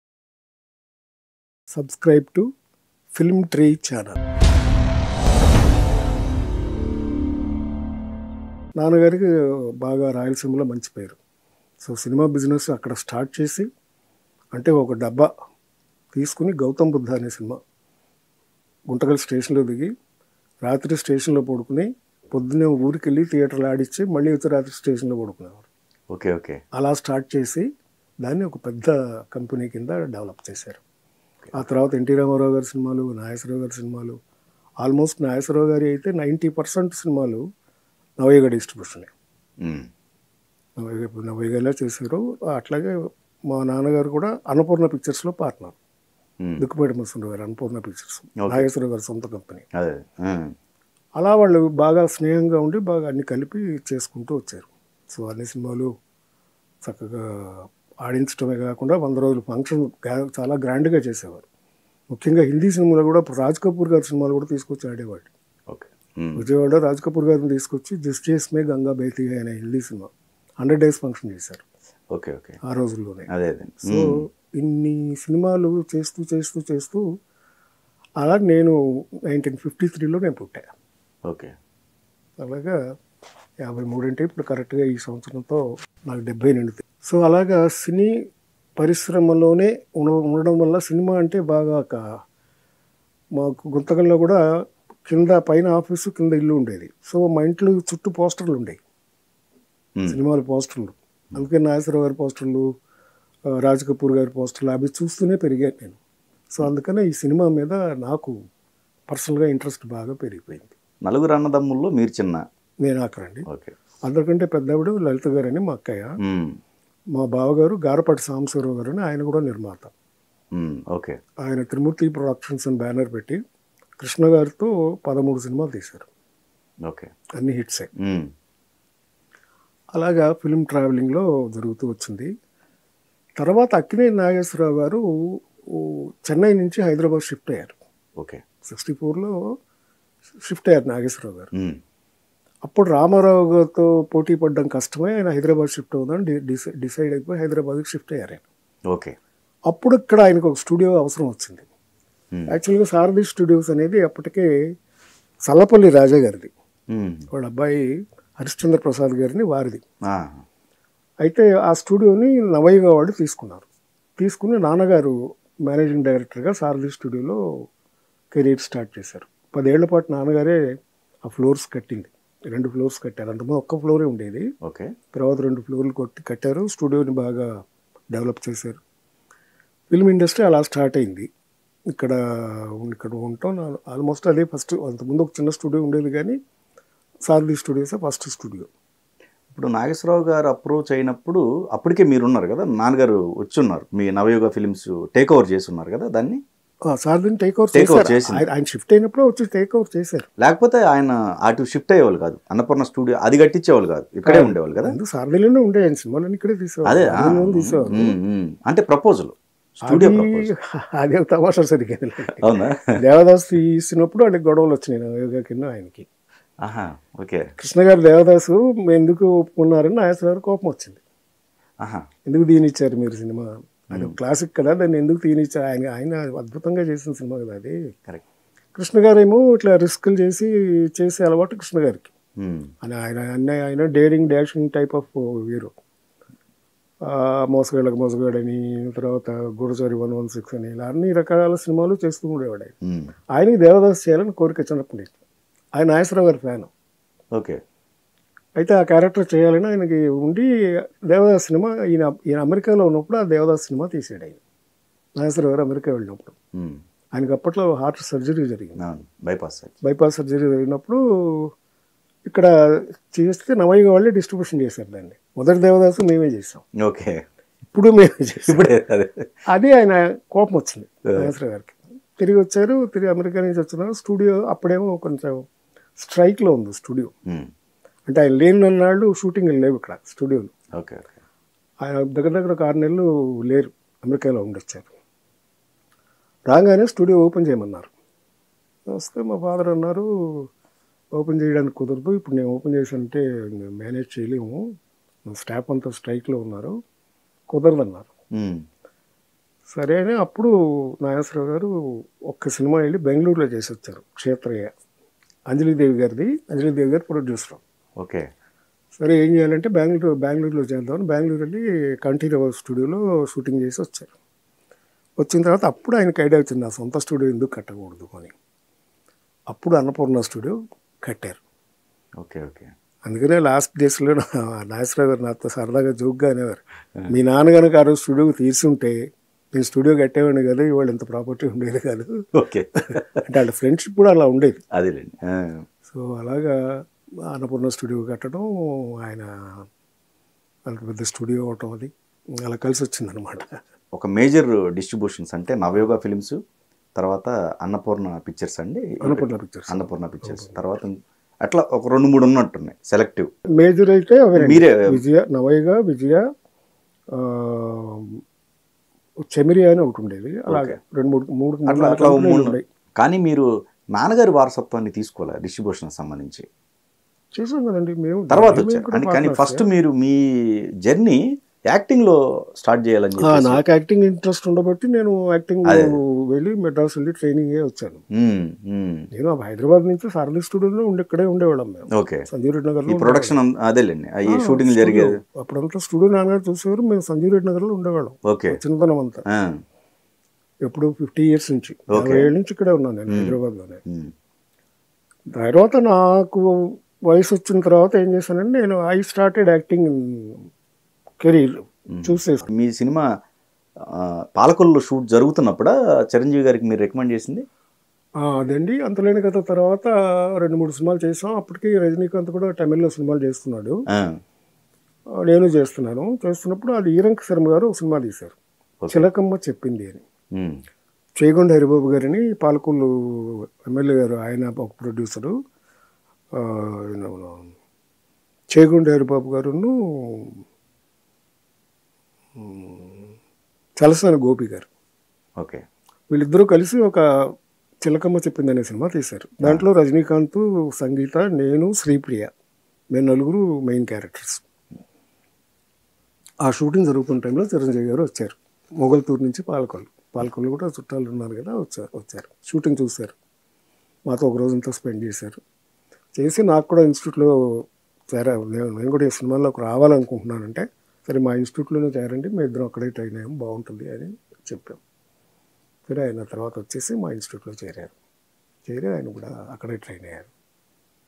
GV Narayana Rao Shocking Comments On ANR And NTR | Real Talk With Anji | Film Tree
Film Tree Channel is Completely Entertainment Hub..